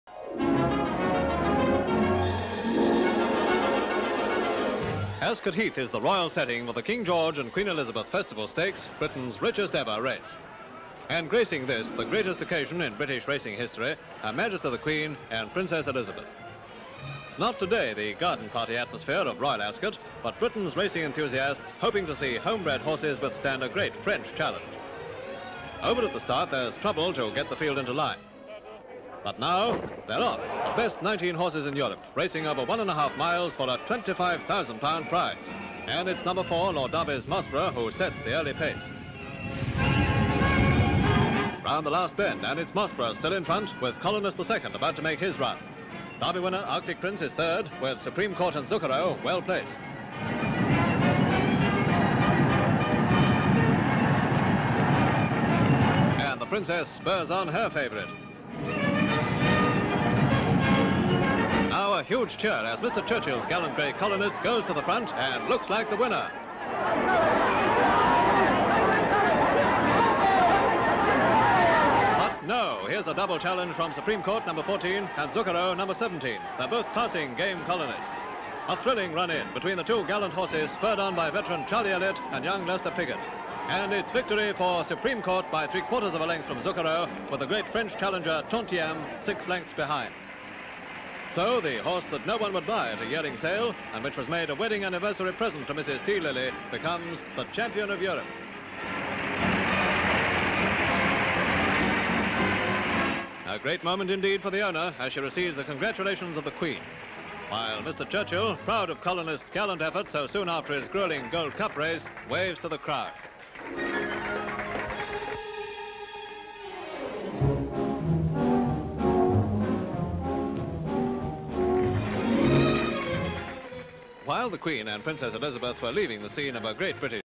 Ascot festival horse races (1951)
Race horses cheered by royalty at Ascot

Full Description:

SLATE INFORMATION: Ascot Festival Stakes

ENGLAND: Berkshire: Ascot:
EXT

ASCOT. Ascot Festival of Britain Stakes won by Supreme Court with Zuccero 2nd .. ridden by Piggott and Elliot inversely.. Colonist II leads for long way.. Attended by Princess Elizabeth Queen Elizabeth Mr Churchill.. Crowds fashions

CHURCHILL WINSTON M.P. At Ascot Festival of Britain Stakes when Colonist II leads for long distance.. beaten to 4th place by Supreme Court Zucchero and Tamtieme

COLONIST THE SECOND. RACE HORSE. Runs in Festival Stakes at Ascott taking lead for long stretch to be beaten into 4th place..winners Supreme Court and Zucchero..Churchill and Queen and Princess Elizabeth at Race

PRINCESS ELIZABETH With Queen Elizabeth at Festival of Britain Stakes Race at Ascot..Cheers her favourite.. wanders thro' crowds

ELIZABETH QUEEN With Princess Elizabeth at Ascot Festival of Britain Stakes Race..wander thro' crowds after

ELLIOT CHARLIE...(British Jockey) Wins Festival Stakes at Ascot on Supreme Court..with Piggott on Zucchero 2nd

PIGGOTT LESTER (Boy Jockey) Rides Zucchero to 2nd place at Ascot in Festival Stakes

Sport; Horseracing

racing, horse races, royalty
Background: Race horses cheered by royalty at Ascot